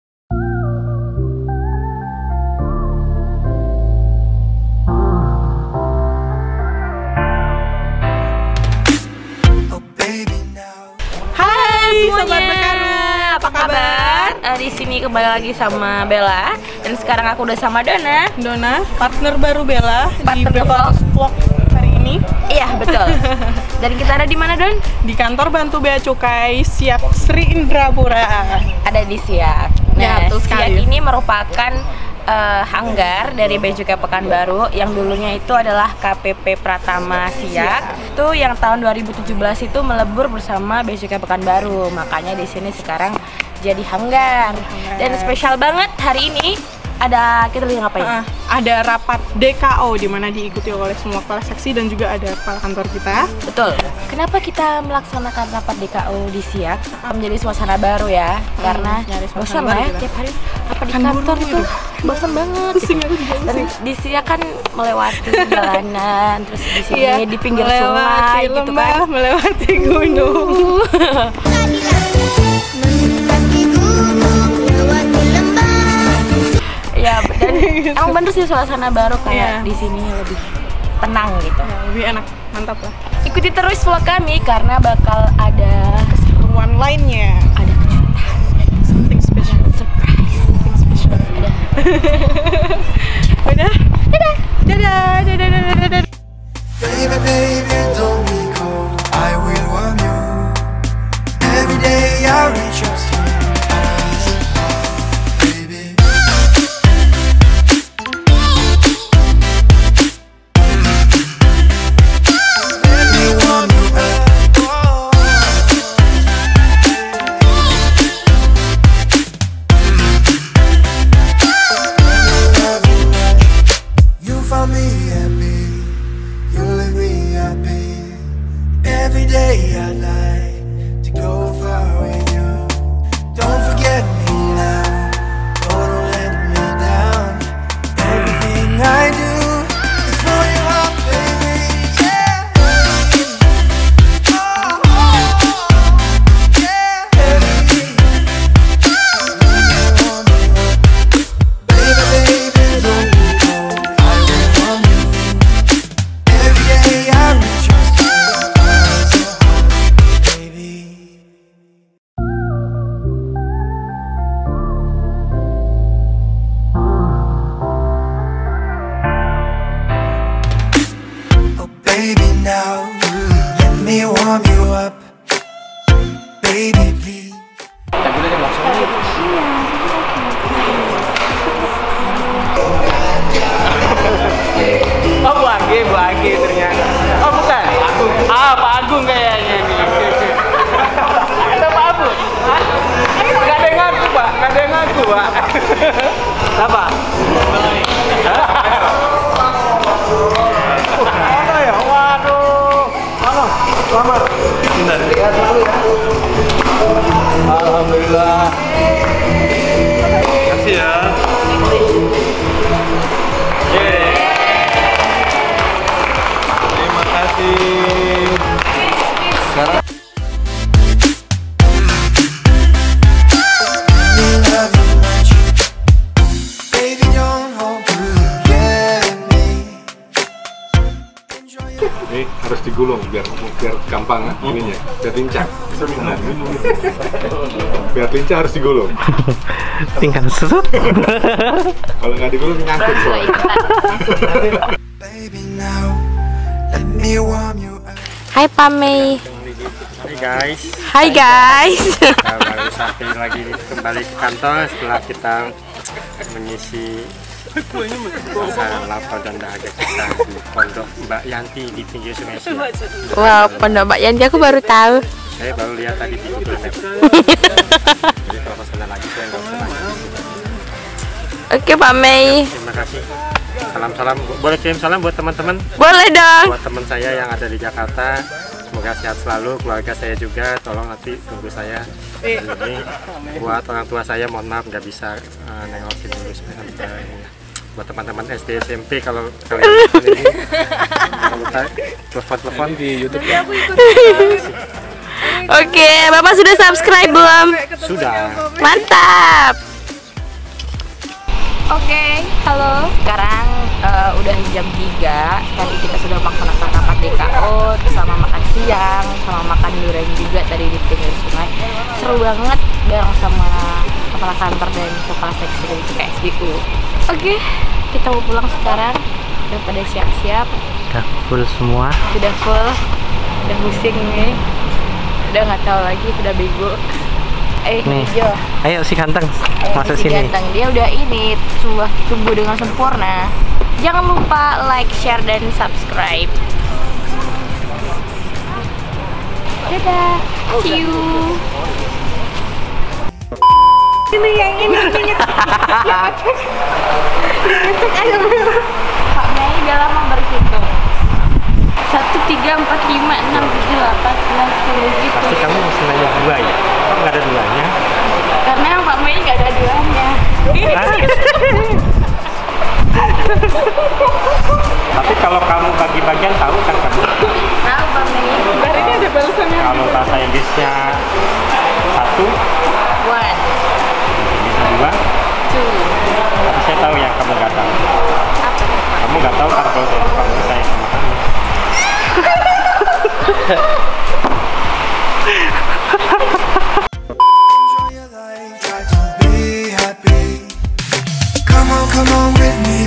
Bekaru's Vlog (10) - Rapat DKO Istimewaa
Rapat DKO Istimewaaaaaaaaaaa
Hallo Sahabat Bea Cukai!
Rapat Dialog Kinerja Organisasi (DKO) merupakan pertemuan Kepala Kantor bersama Kepala Subbagian Umum dan Kepala Seksi untuk membahas kinerja selama sebulan yang lalu dan sebulan yang akan datang. Kali ini rapat dilaksanakan di Kantor Bantu Bea Cukai Pekanbaru di Kabupaten Siak Sri Indrapura loh, untuk menemukan suasana baru dan pemikiran yang lebih segar. Dua jam lebih perjalanan dari Kota Pekanbaru diwarnai dengan hutan sawit dan jalanan yang ekstrim. Tapi sangat seru, karena di Siak banyak sekali harta karunnya loh. Hihihi
Bertepatan dengan Rapat DKO, salah satu kepala seksi ada yang lagi ulang tahun looh.